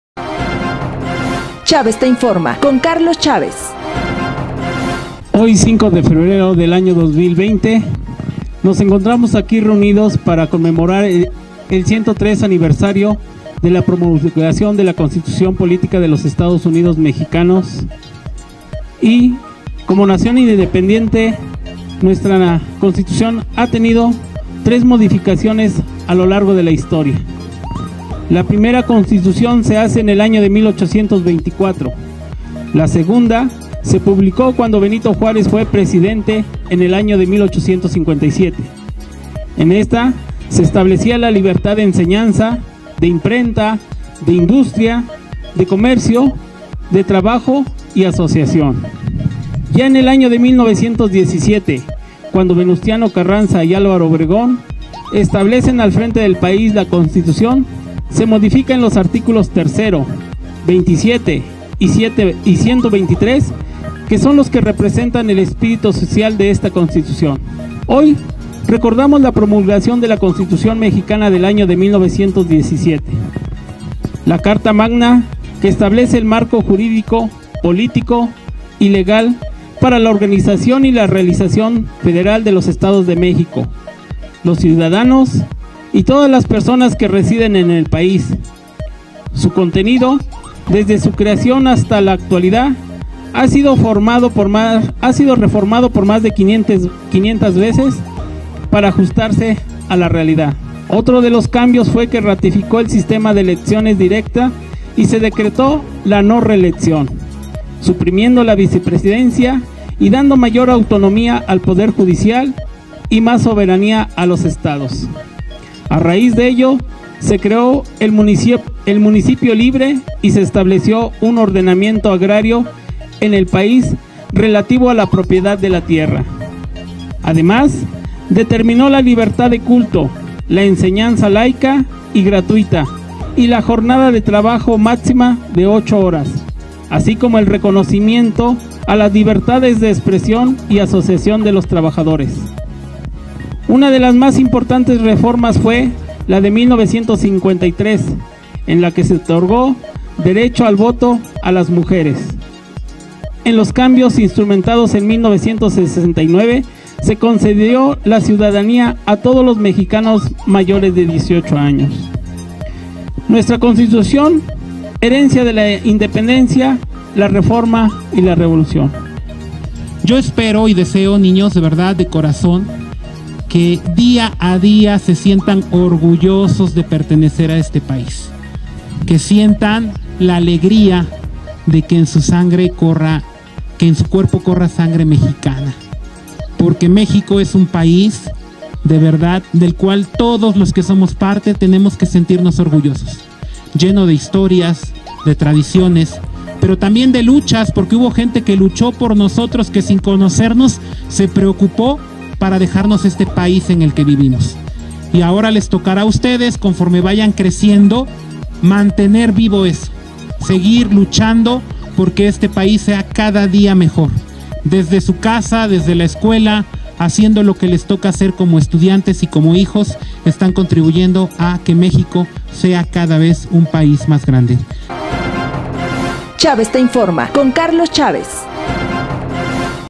CEREMONIA 103 ANIVERSARIO DE LA PROMULGACIÓN DE LA CONSTITUCIÓN MEXICANA.
CEREMONIA 103 ANIVERSARIO DE LA PROMULGACIÓN DE LA CONSTITUCIÓN POLÍTICA DE LOS ESTADOS UNIDOS MEXICANOS.

Ceremonia realizada en el parque de Chalchicomula de Sesma.